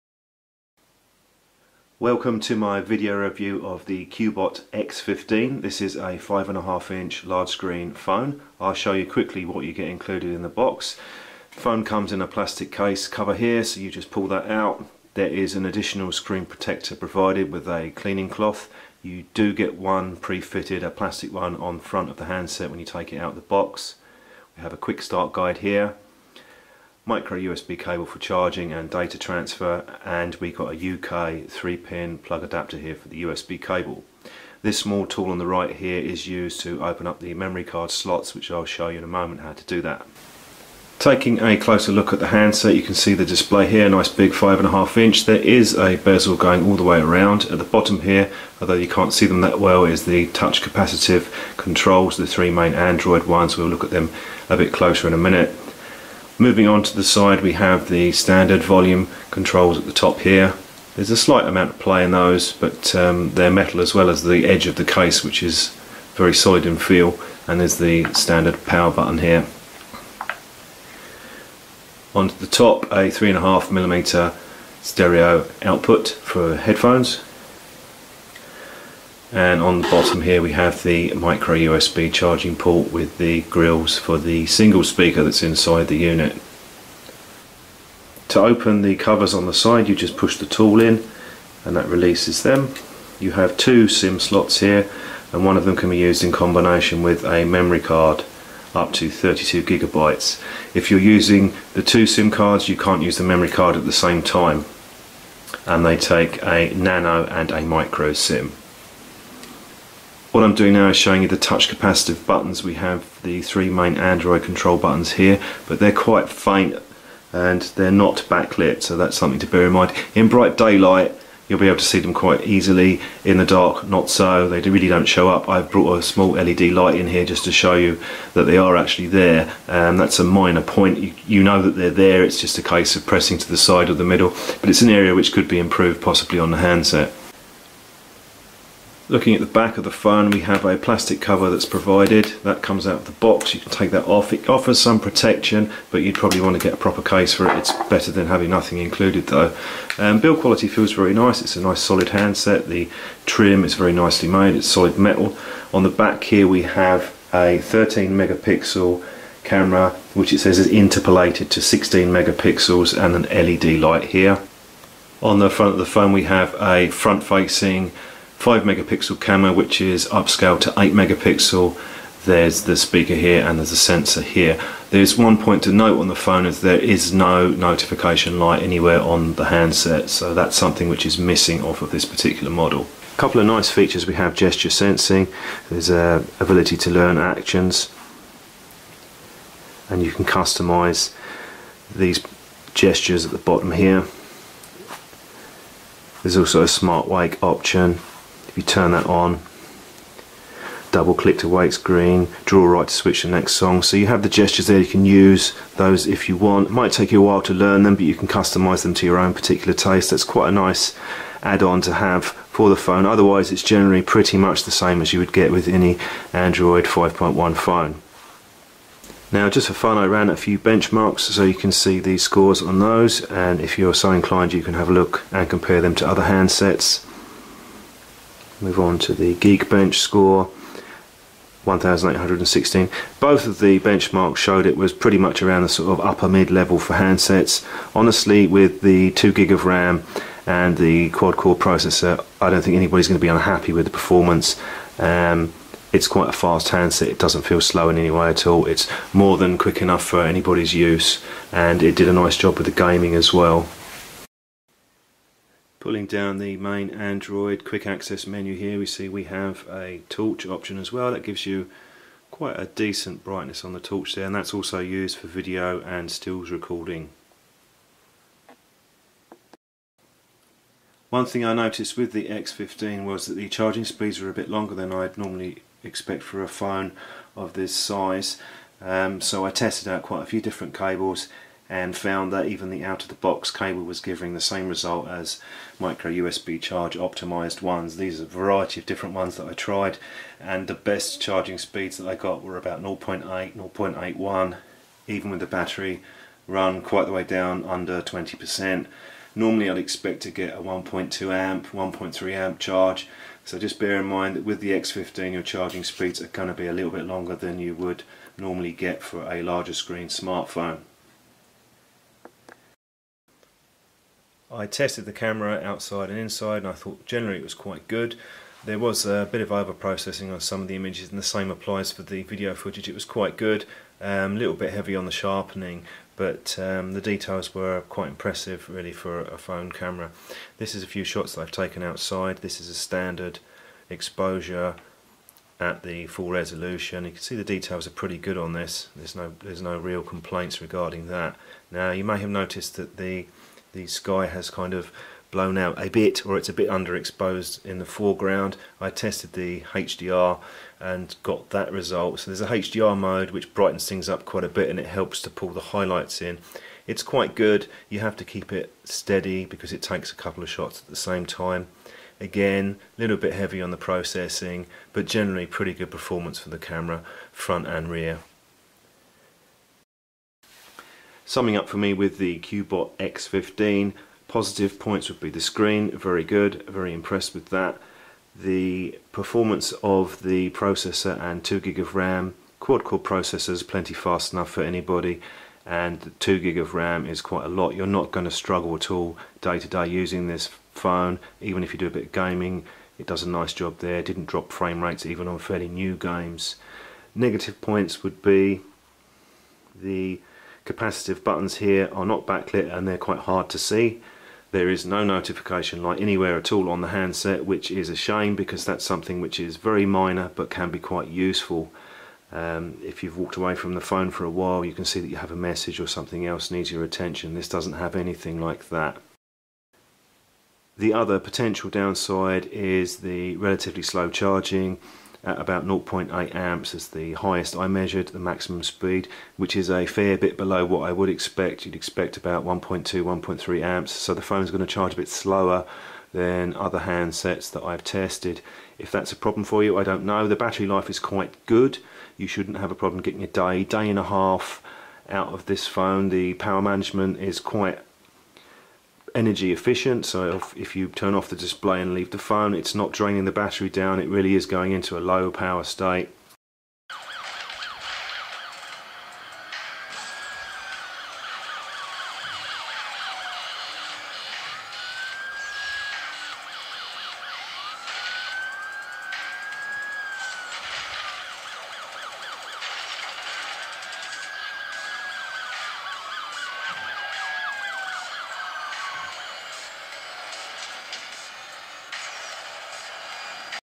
Cubot X15 Review
Hands on with the latest flagship big screen phone from Cubot

Main Specs:
Model:CUBOT X15
Band
GSM:850/900/1800/1900MHz
WCDMA:900/1900/2100MHz
FDD-LTE:B1/3/7/20
SIM Card:Dual SIM Card Dual Standby (1 Micro SIM card slot&1 Nano Sim card slot)
Service Provide:Unlocked
Style:Bar
Color:Glod
Shell Material:Plastic back shell,metal frame

System
OS:Android 5.1
CPU:MTK6735 1.3GHz 64Bit Quad-core
GPU: Mali-T720
ROM:16GB
RAM:2GB

Screen
Display Size:5.5 Inch
Type:IPS-NEO, multi point touch screen
Resolution:1080×1920 pixels

Camera
Back Camera:16.0mp dw9714 graphic sensor, 5P lens, ƒ/ 1.8 aperture
Front Camera:8.0mp, 5P lens , ƒ/1.8 aperture Intelligent beautyDynamic background automatically hide

Battery Type:Lithium
Capacity:2750mAh
Earphone Port:3.5mm
Card Extend:Support Micro SD(TF) card up to 32GB extended

Data transfer:USB/Bluetooth 4.0
Mobile internet:WAP/WiFi

General
Language:Multi-Language:Support
Phonebook:1000
Message:SMS/MMS
Input:Handwrite/Keypad
TV:No
OTG:Yes
GPS:Yes,support AGPS
JAVA:No
WIFI:Yes, 802.11 b/g/n
Bluetooth:v4.0
Gravity Sensor:Yes
Air-control:Yes

Summary of review:

Areas I liked:
+ Superb IPS 5.5” screen with high resolution 400 PPI density display contrast is great and good viewing angles too.
+ Runs Android 5.1
+ Nice build, metal edge casing and slim profile at just 7mm thickness
+ Plenty of performance from the quad core 64-bit MediaTek MT6735, 2GB of ram and 16GB storage spec is well above many big name phone brands at this price
+ Front 5mp and rear 13mp cameras (both are interpolated at 16mp and 8mp just leave them at their native resolutions) are quite good plenty of details and deliver decent images/video
+ Gaming performance is strong with a Mali-720 GPU even on more demanding games it delivers a smooth experience
+ 2750mAh battery should easily last most users a full day a couple of days is possible with lighter use
+ Energy efficient in standby mode with the screen off it consumes very little power
+ Wi-Fi held a signal better than most handsets
+ Excellent GPS accuracy
+ OTA updates (over the air)
+ Support for “gestures” unlocking

Weaker points:
- Navigation buttons are hard to see in low light, and not backlit
- Screen somewhat reflective outdoors
- Memory card must be removed to use both sim cards
- No notification light, single speaker (not dual)
- Battery charging is “modest” at 0.80 amps most phones can charge at 1.2/1.3 amps expect extended charging times
- Images and video a bit over processed particularly sharpening (can be reduced/improved by tweaking settings or using other camera apps)

Notes:
Sim cards: One Nano and one Micro Sim type
4G support extends to LTE 800/1800/2100/2600MHz you might want to check your provider to ensure it works fully
Micro SD card up to 32GB
Out of the 16GB storage around 10.5GB is available for use